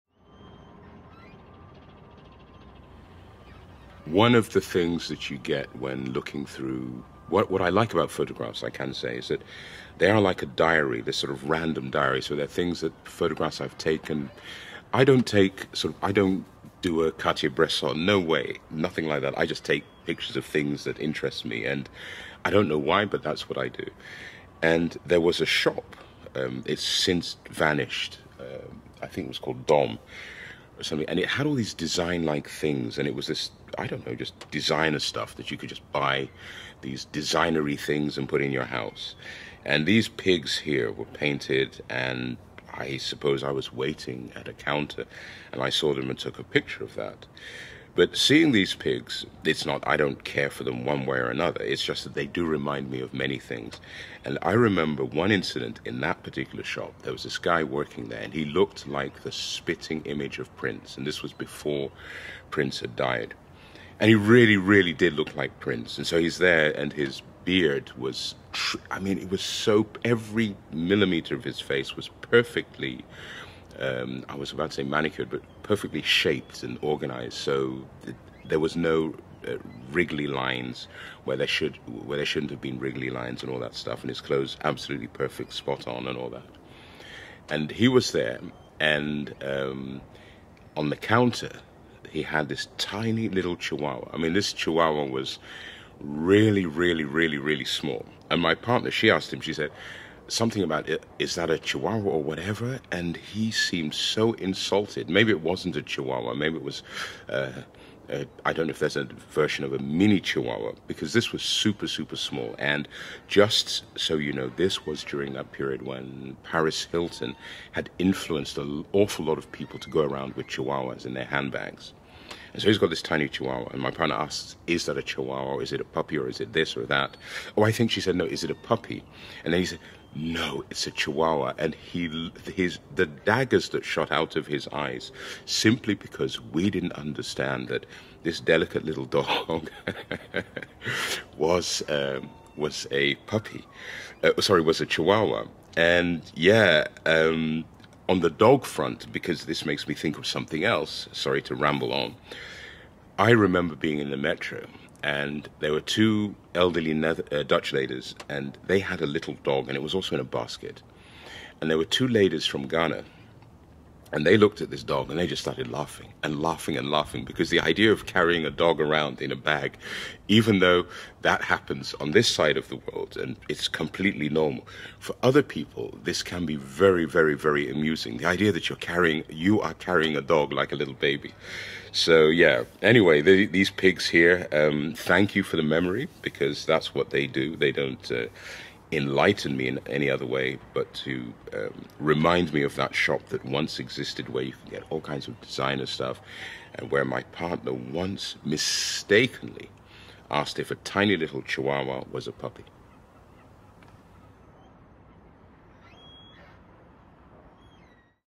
Amsterdamica S05E35 – Oh I Remember That
A couple of little standing on the shelf, and suddenly I remember things that have come and gone – such as that time when every other person seemed to walk around with a chihuahua in their bag!